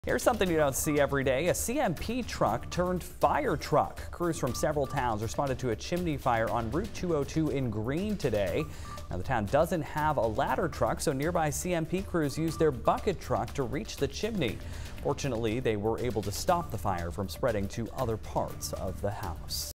Fire crews use bucket truck to reach chimney during blaze in Greene
The town doesn't have a ladder truck, so nearby CMP crews used their bucket truck to reach the chimney.